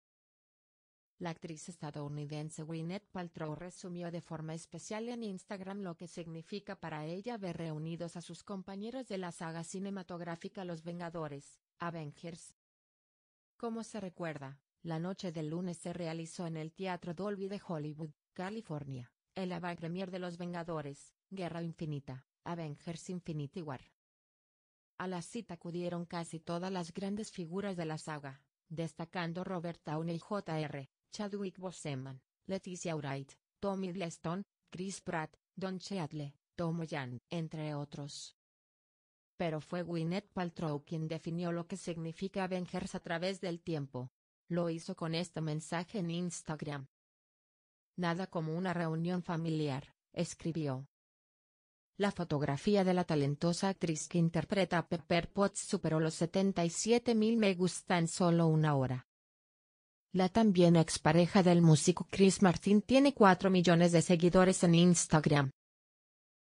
Instagram: Gwyneth Paltrow y su tajante postura sobre premiere de "Infinity War"
La actriz estadounidense Gwyneth Paltrow resumió de forma especial en Instagram lo que significa para ella ver reunidos a sus compañeros de la saga cinematográfica "Los Vengadores" (Avengers).